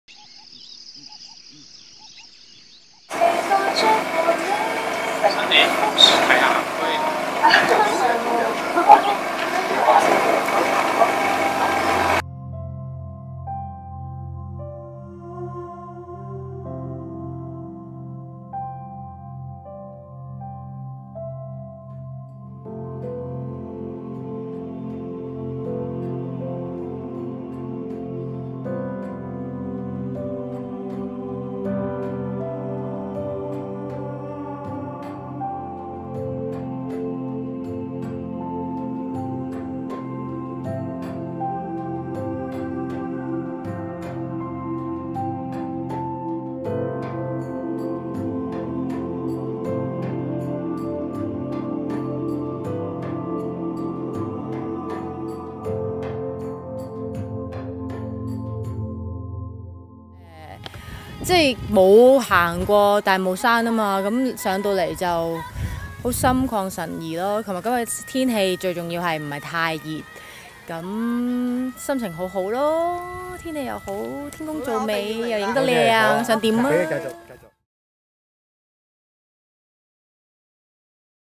容祖兒 Joey Yung | 對我來說，side track其實都是main track | ELLE HK
我們都喜歡Joey，喜歡看到她傻笑、吐舌、自問自答；也喜歡看到她很認真、很努力按攝影師要求擺各種pose。雙子座的她，既樂天又沉靜、既調皮又認真。跟她聊天，有時會被她帶到很遠，很遠，卻是笑聲不絕，很能感受到她的真摯。